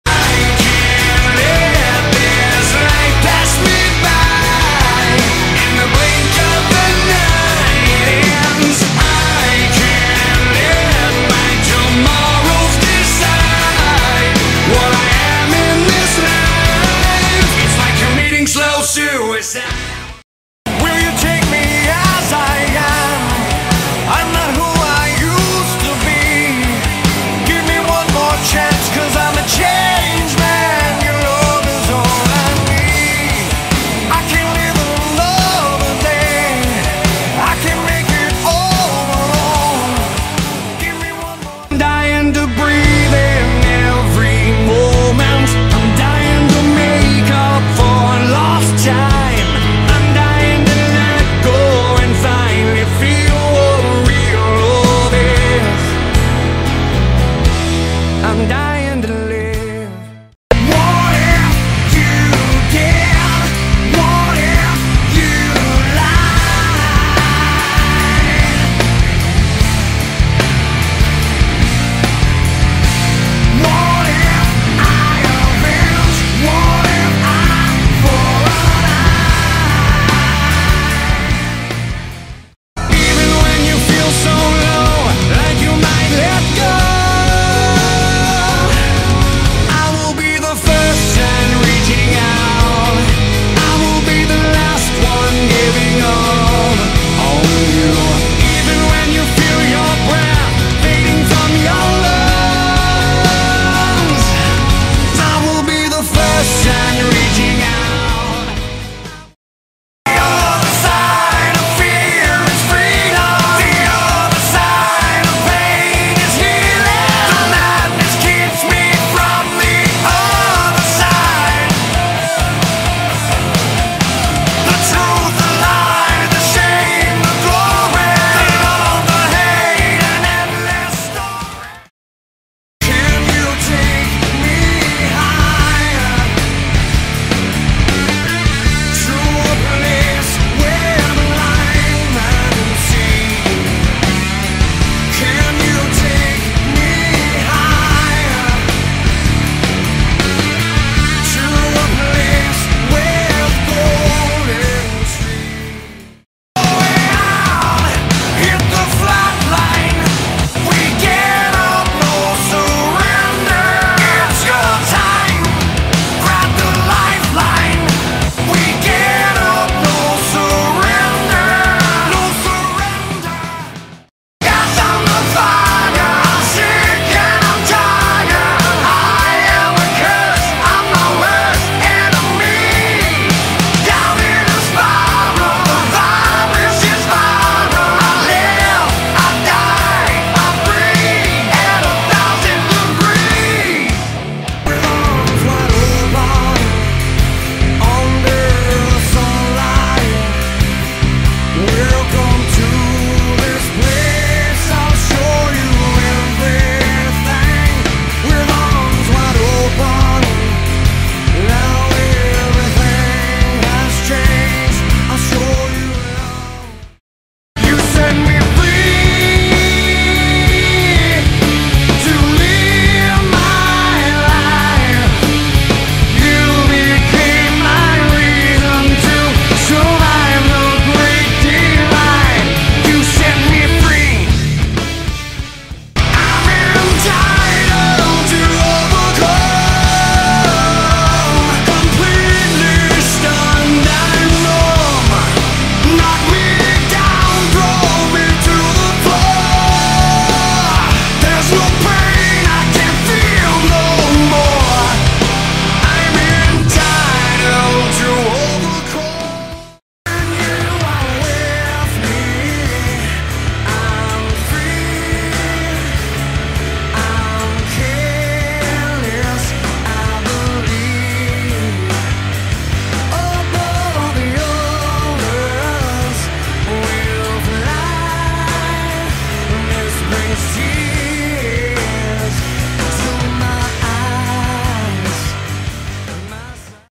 My favorite songs sung my Scott Stapp (Creed, Art of Anarchy)
Scott Stapp has one of the most iconic voices in rock music and is also one of my favorite singers. It has been 20 years since Creed's debut album and he is still singing and releasing great music. Keep on rocking, man.